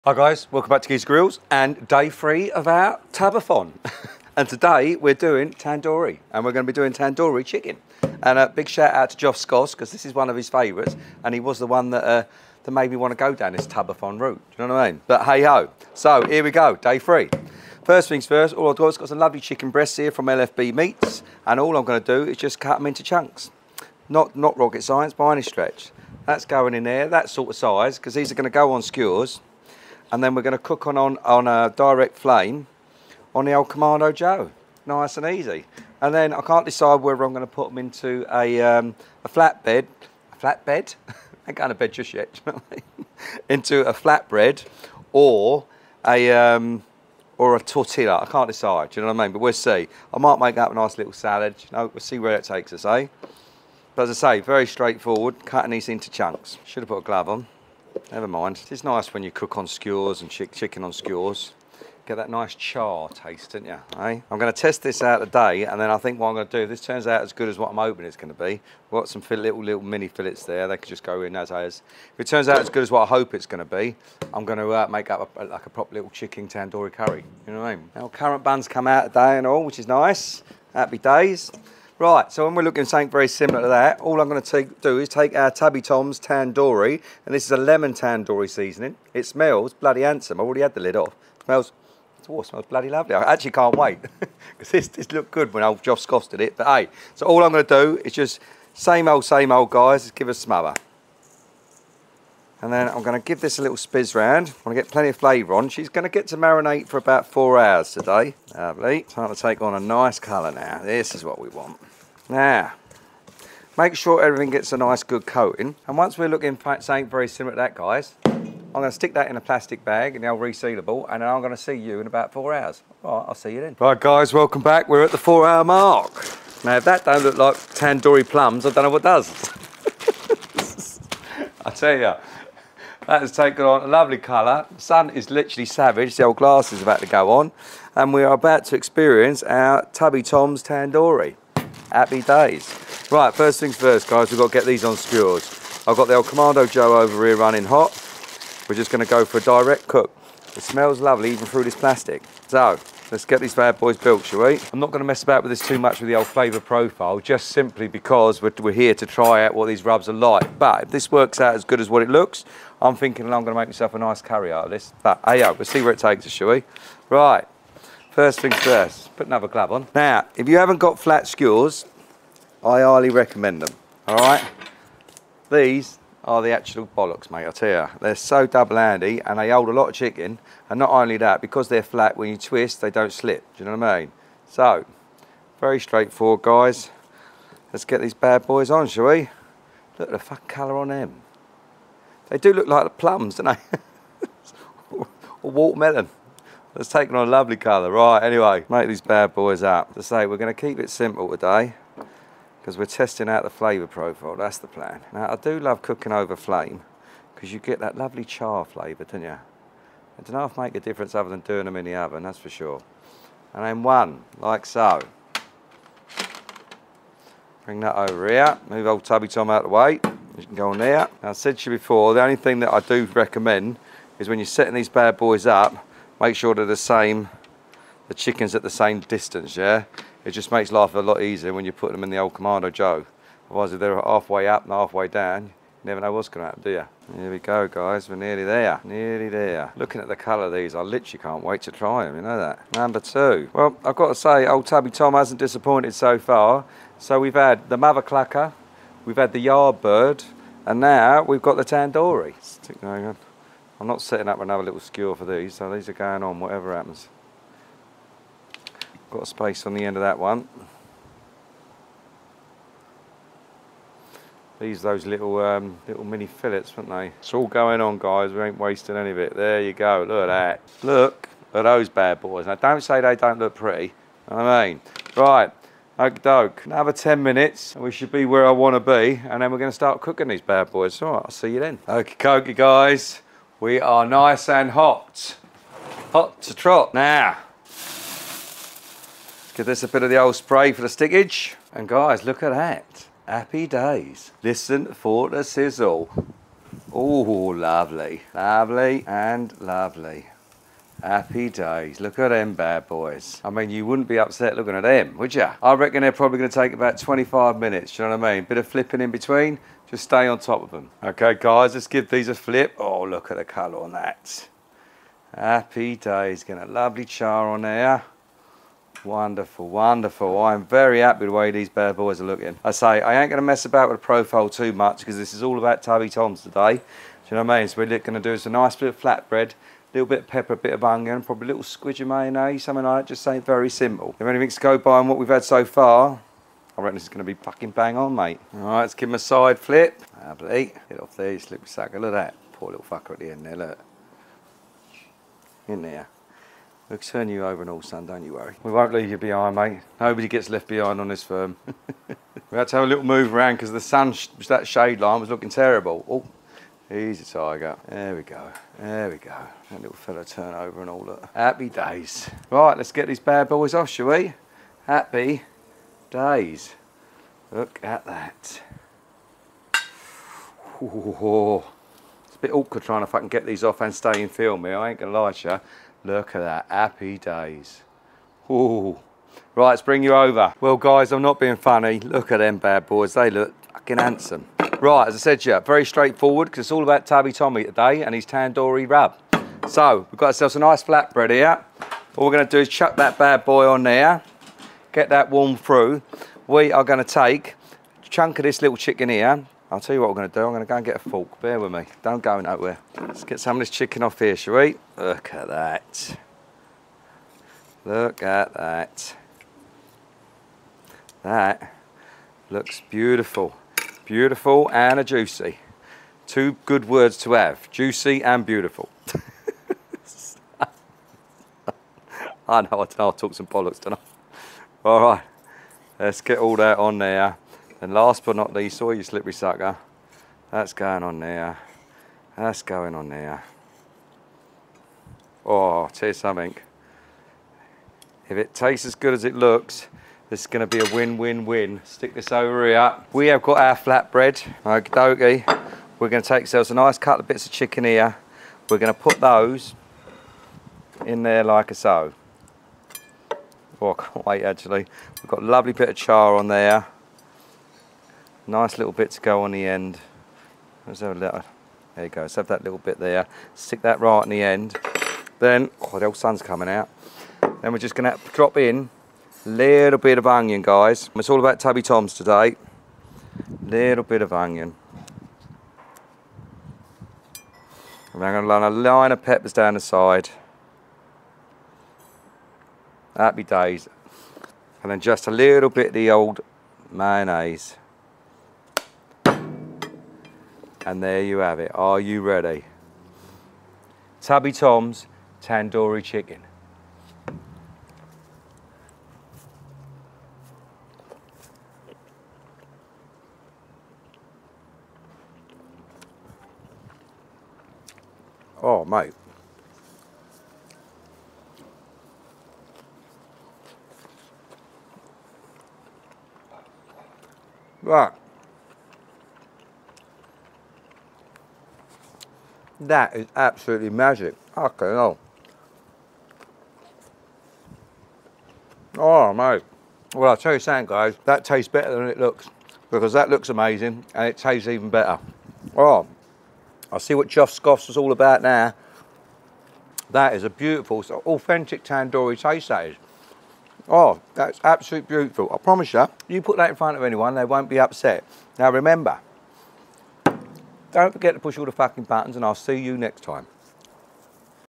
TUBATHON TUBBY TOM TANDOORI CHICKEN
TUBATHON TUBBY TOM TANDOORI CHICKEN COOKED ON THE KAMADO JOE THEN SERVED IN A FLAT BREAD WITH PEPPERS AND ONION TOPPED OFF WITH MAYO HAPPY DAYS